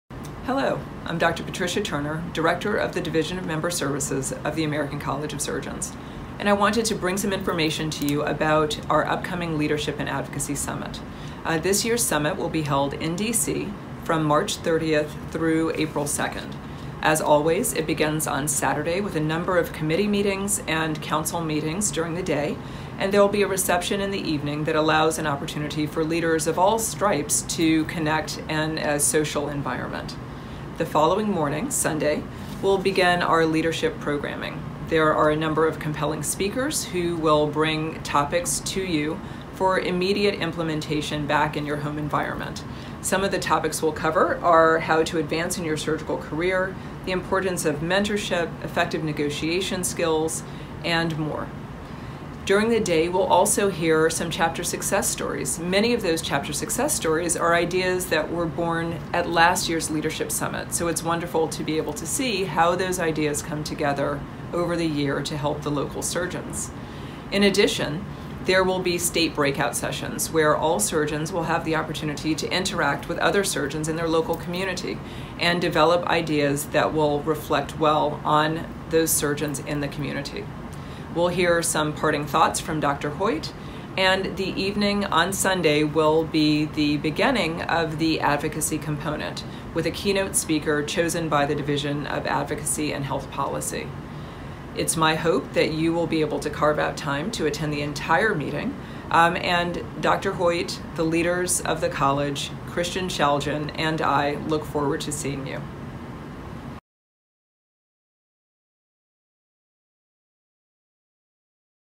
Why Attend the 2019 ACS Leadership and Advocacy Summit
ACS Member Services Director Patricia Turner, MD, FACS, highlights some of the many reasons you should attend the 8th annual ACS Leadership and Advocacy Summit, March 30 - April 2, in Washington, DC.